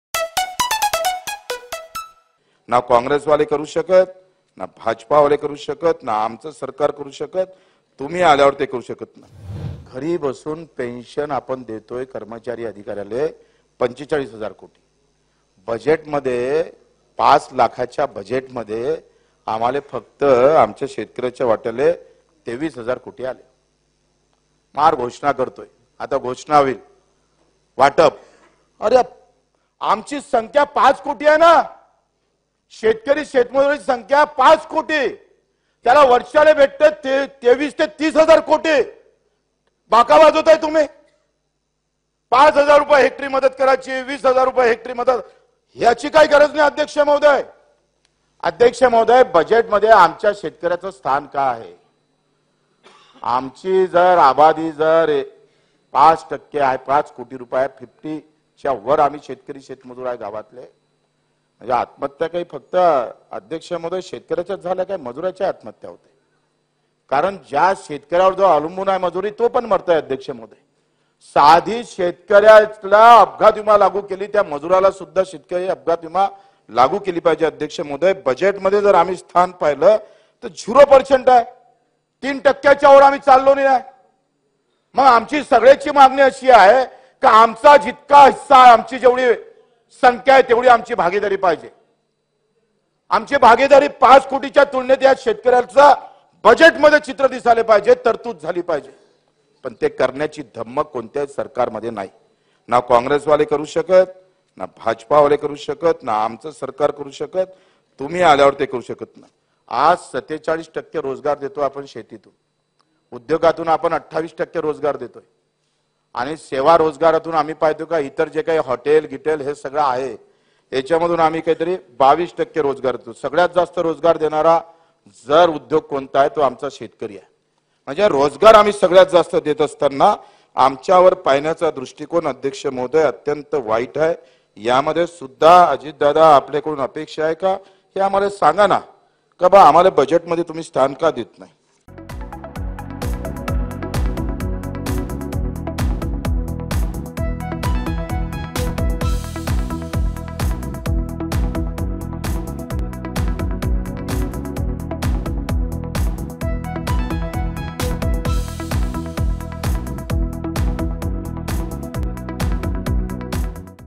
Eknath Shinde यांचं सरकार असो की दुसरं, Bachchu Kadu विधानसभेत म्हणाले | Assembly Session 2023
नागपूर येथे हिवाळी अधिवेशन सुरु आहे, यावेळी बच्चू कडू यांनी शेतकर्यांच्या मुद्दावर भाषण केलं, यावेळी मुख्यमंत्री एकनाथ शिंदे, PM नरेंद्र मोदी, कृषीमंत्री अशा अनेकांना टोला लगावला.